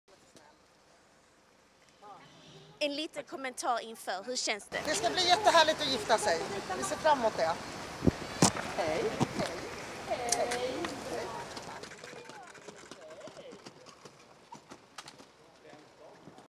Kärleksparet Dominika Peczynski och Anders Borg : "Ska bli jättehärligt att gifta sig"
Drygt ett år efter förlovningen är det dags – under lördagseftermiddagen gifte sig Dominika Peczynski och Anders Borg i en judisk ceremoni i Stockholm.
Inför de 170 gästerna – däribland Fredrik Reinfeldt och Annie Lööf – sa kärleksparet ja till varandra.